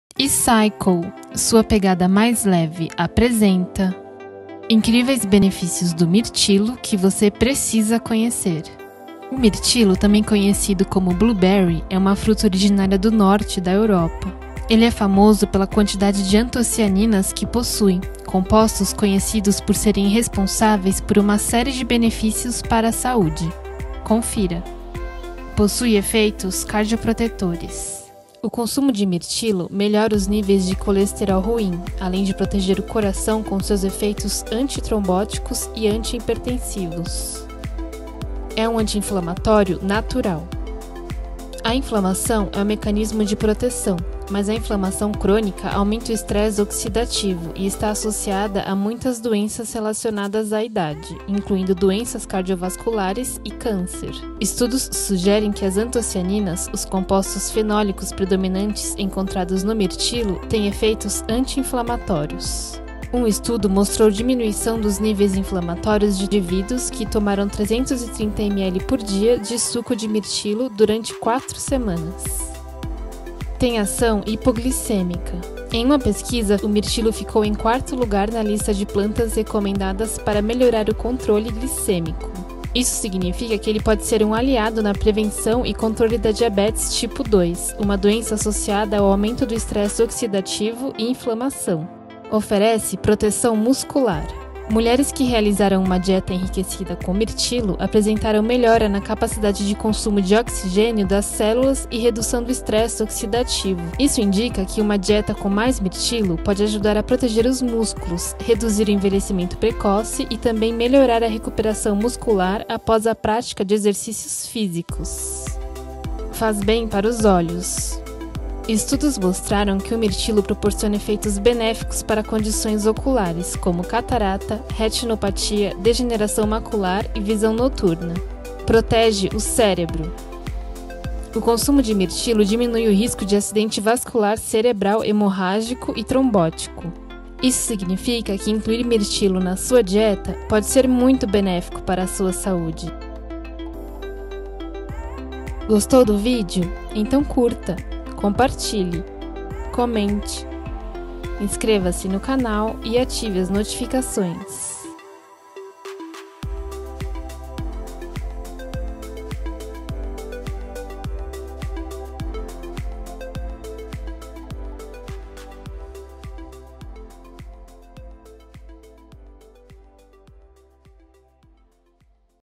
Benefícios do mirtilo para sua saúde
O mirtilo é uma das fontes naturais mais ricas de antocianinas, substâncias que possuem efeito anti-inflamatório e ajudam na prevenção de câncer. Esses componentes são os responsáveis pela cor azul do mirtilo e também o que confere à fruta uma alta quantidade de antioxidantes.

00:26 - Efeitos Cardioprotetores
00:39 - Anti-inflamatório Natural
01:15 - Tem Ação Hipoglicêmica
01:33 - Proteção Muscular
01:56 - Faz Bem Para Os Olhos
02:09 - Protege o Cérebro

Créditos

Roteiro e locução: Stella Legnaioli
Edição, animação e seleção de mídia: Larissa Enohata